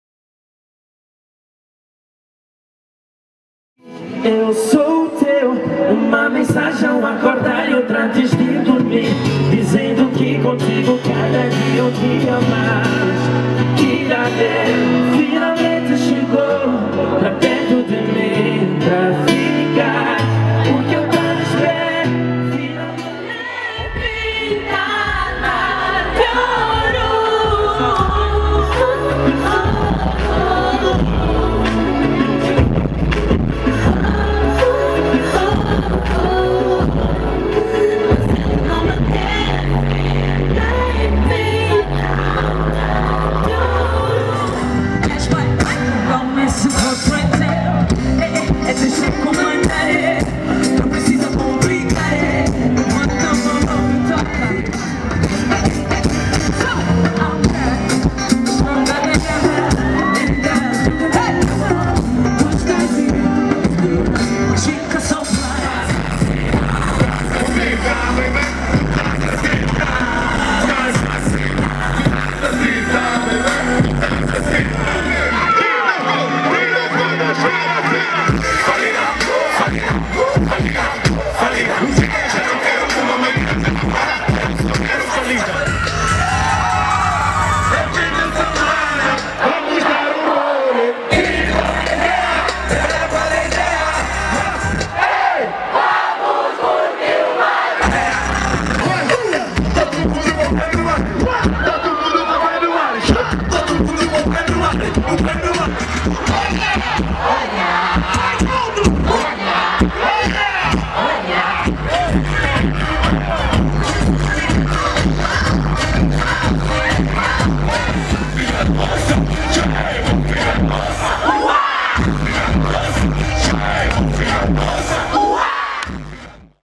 Show Unitel "Festa da Música" 2016
Actuações de Ana Joyce, Pérola, C4 Pedro, Kalibrados, Zona 5